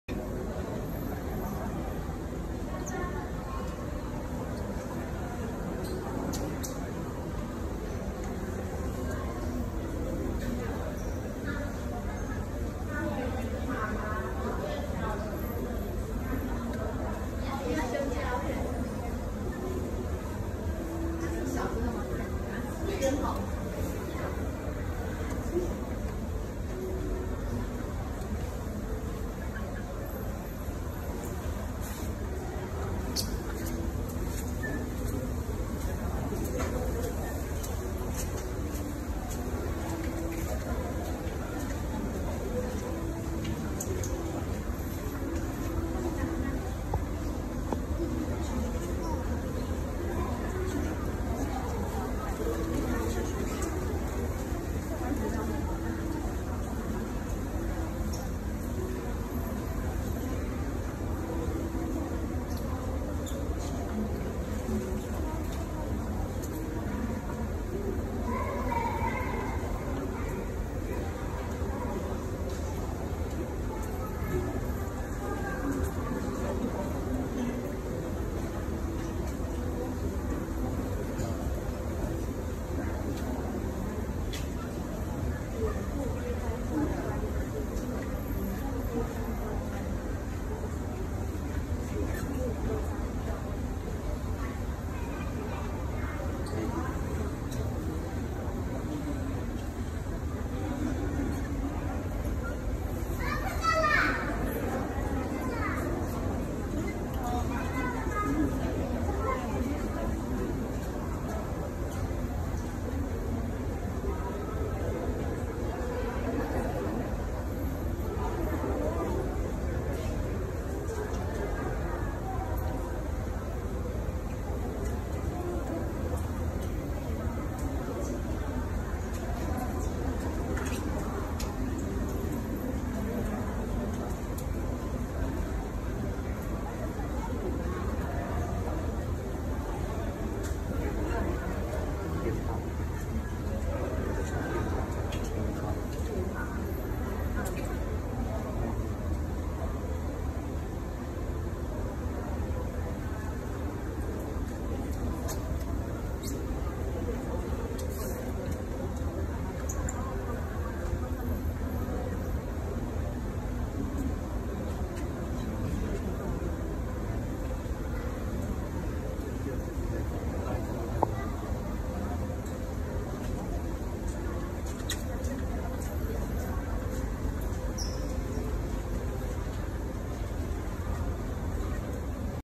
豪宅玩具~3003~3/30(六)功夫熊貓4臺北市立動物園 大貓熊Ailuropoda melanoleuca
為何要在同個地方一直繞圈圈!?
是不是憂鬱阿?!